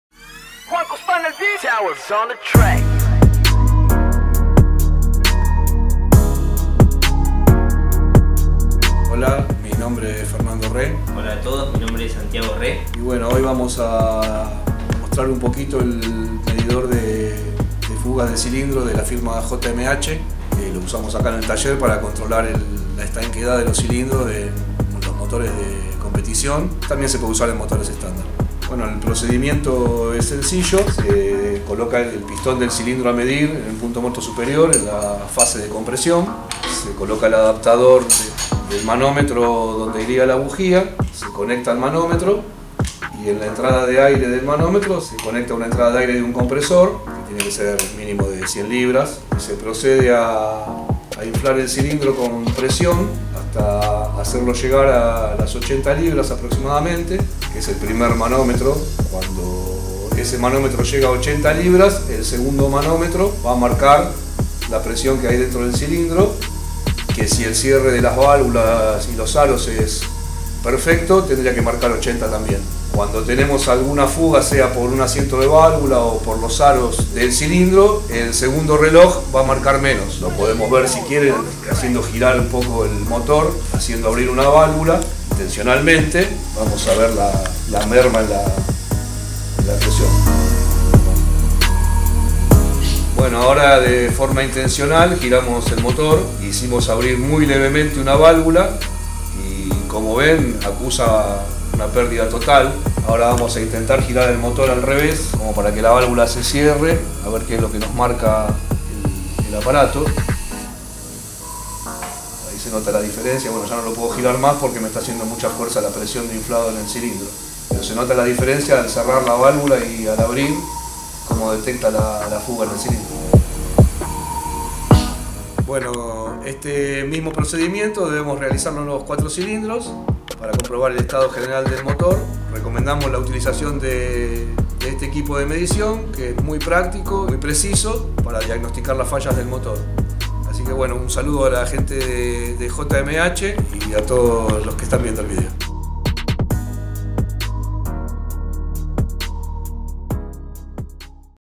Detector de fuga de compresión x la Familia Re
Fernando y Santiago Re motoristas de categorías nacionales y zonales te explican como usan el detector de fuga de compresión, un producto de fácil utilización y con grandes resultados.
Gracias Familia Re por el excelente trabajo.
Gracias @garbisufotografia por la gran labor con imágenes y video.
Gracias @fgrcompeticion @fernandoremotores @santiagorecompeticion